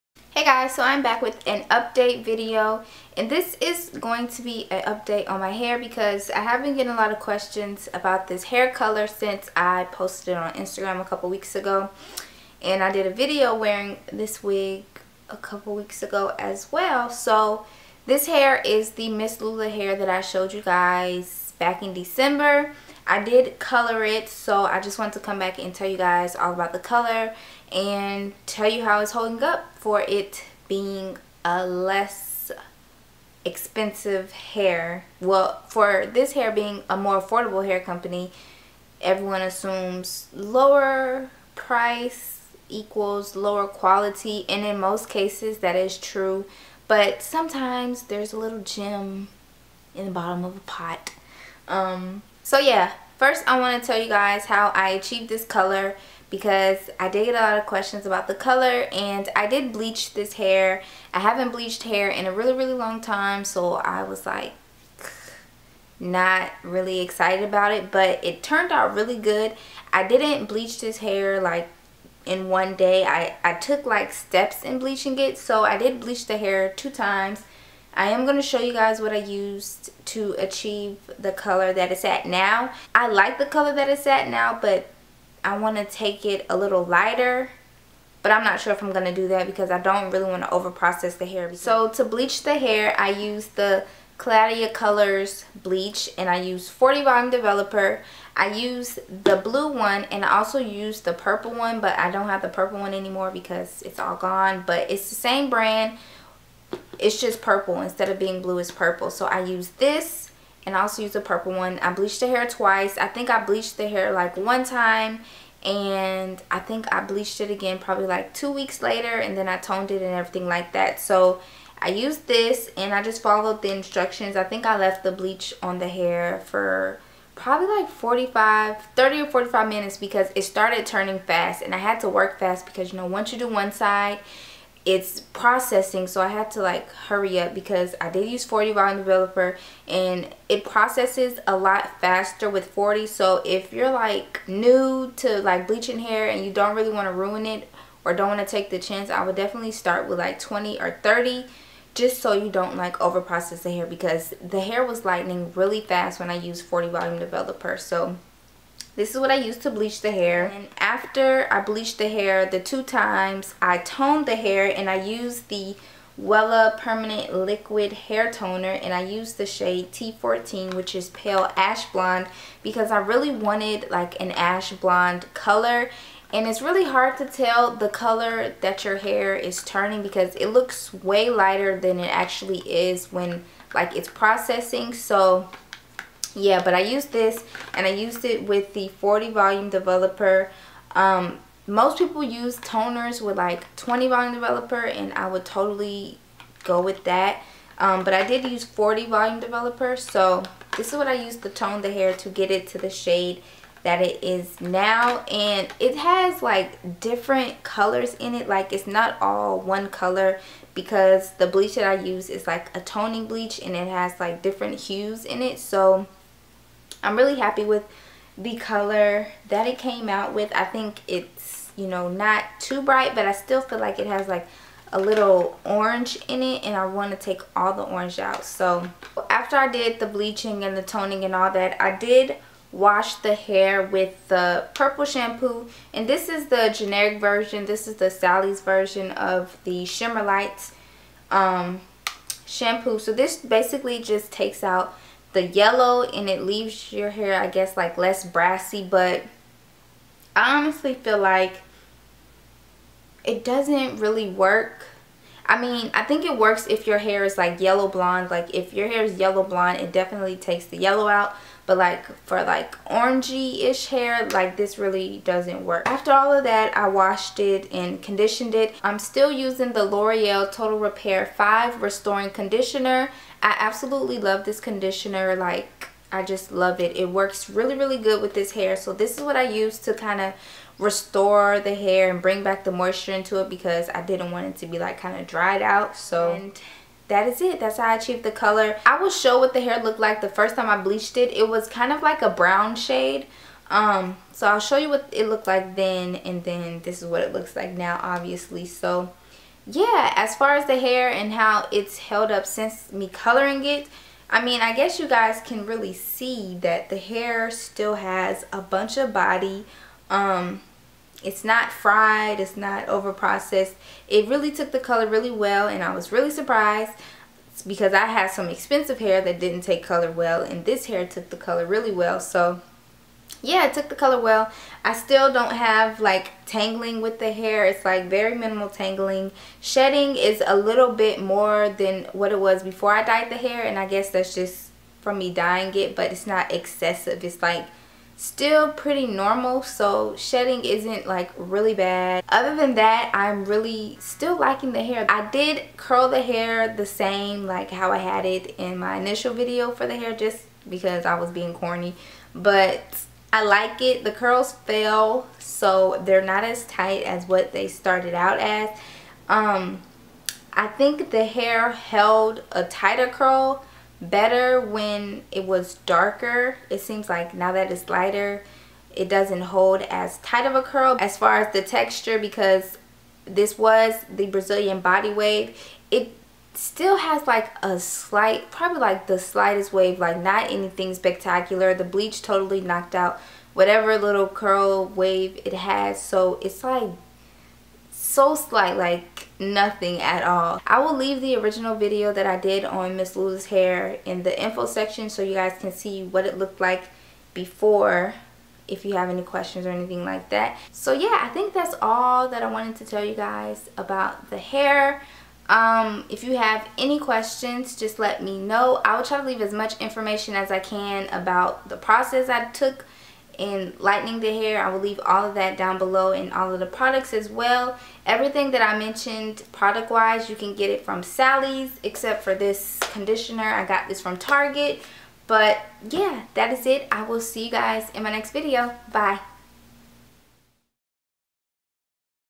Summer Color! Bleached MS Lula hair Brazilian virgin hair! Thanks
Thanks Lakiastar for reviewing our hair! Great color for this summer look! You deserve it!